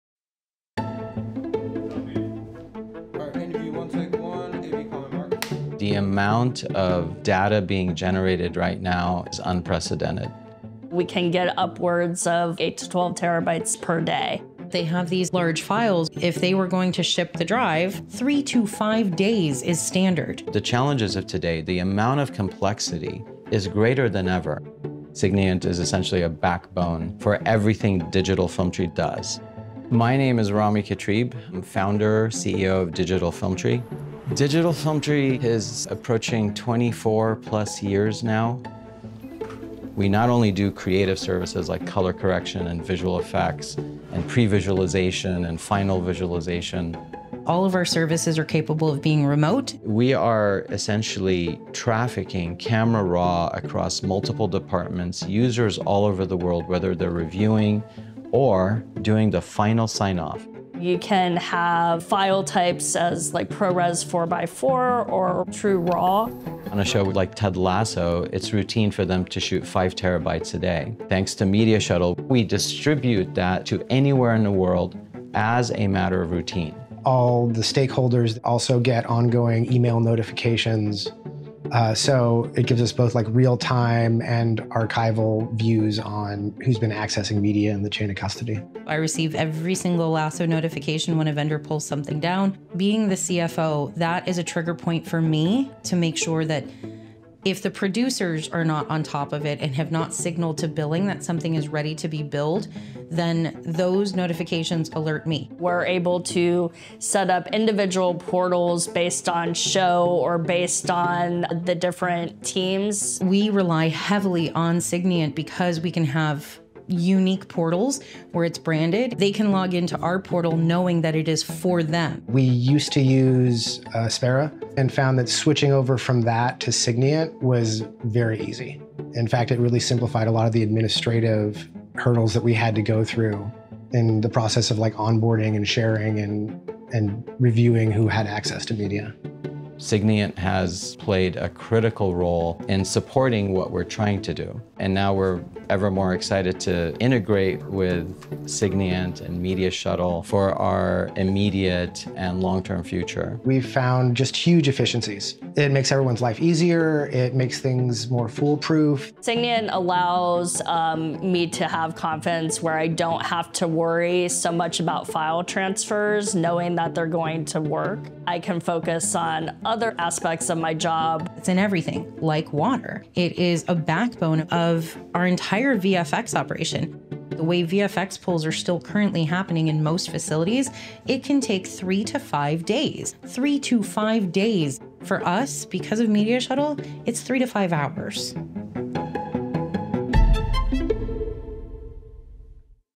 DigitalFilm Tree — a SIGNIANT case study
Explore how the Signiant Platform streamlines post-production workflows, making it easy to manage large volumes of data efficiently. Learn from industry experts at Digital Film Tree as they discuss handling terabytes of data daily and the challenges of modern media operations.

L.A.-based post-production facility DigitalFilm Tree uses Signiant Media Shuttle to provide theatrical films, documentaries, and scripted and unscripted television series with dailies, VFX, color, and finishing services for shows like "Ted Lasso," "Shrinking," "The Umbrella Academy" and "Manifest."

Watch how Signiant Media Shuttle enables fast, reliable file transfers, real-time notifications, and centralized control, transforming operations for shows like "Ted Lasso."  Media Shuttle is trusted to distribute DigitalFilm Tree’s massive, valuable media anywhere in the world.

“It is a backbone of our daily operations,“ said Nancy Jundi, DigitalFilm Tree COO and CFO. “Media Shuttle, for us, is incredibly ubiquitous. It’s in everything, like water.”